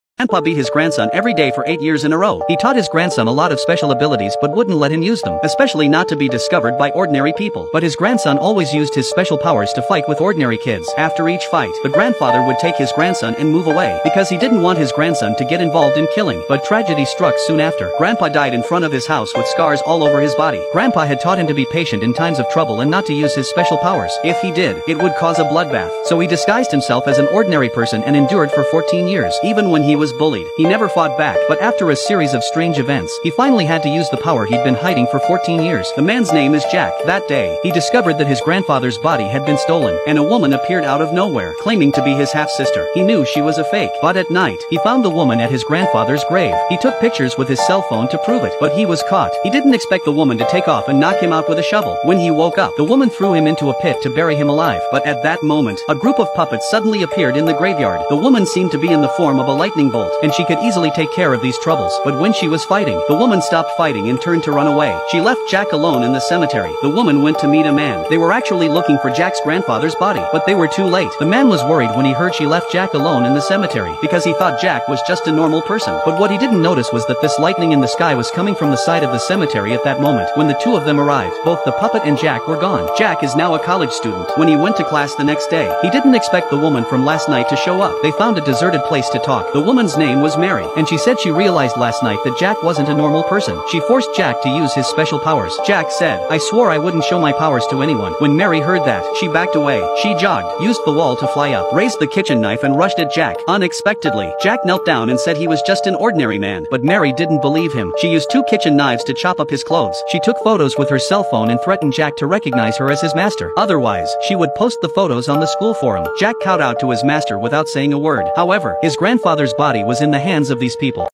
The boy grew up with superpowers, but he could only pretend to be normal 😱
Zhang Chulan was a typical college student until he became involved in a horrific occurrence while visiting his grandfather's tomb in a small village. Chulan is attacked by zombies while strolling through the graveyard to see his deceased grandfather, until a mysterious female carrying a knife emerges and takes him to a showdown with the zombies. Chulan's life begins to change after successfully repelling the zombies and encounters with the knife-wielding girl known as Feng Baobao, as he learns that the martial arts technique taught to him by his grandfather, known as Qi Ti Yuan Liu, is a technique sought after by many martial arts groups with ulterior motives.